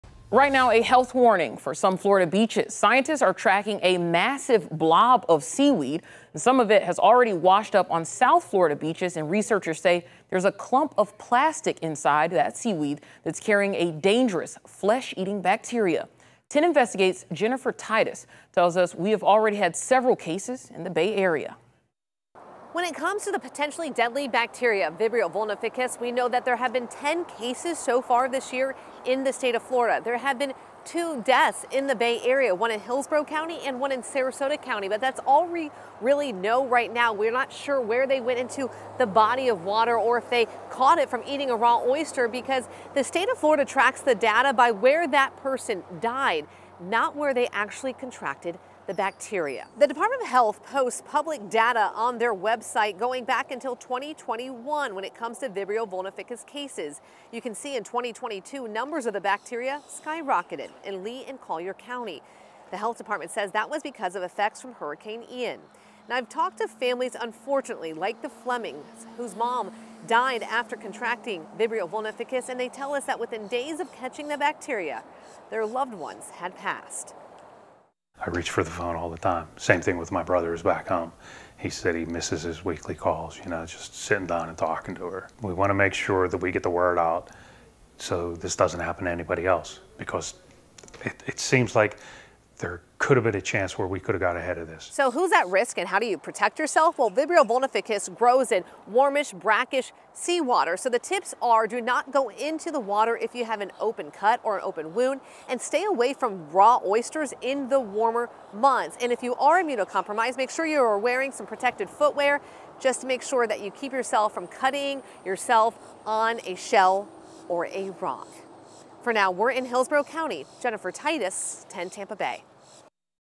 Researchers: Seaweed blob washing up on Florida beaches carrying flesh-eating bacteria
For anyone who is planning on taking a beach trip, you might want to be cautious of the water if you see any seaweed on shore – some might contain flesh-eating bacteria.

The colossal seaweed blob, drifting from the Atlantic Ocean, is starting to show up on some Florida beaches. Researchers say it could stick around for months.

The clumps of brown seaweed, also called sargassum, are part of a massive bloom collecting in the Caribbean Sea spanning nearly 5,000 miles.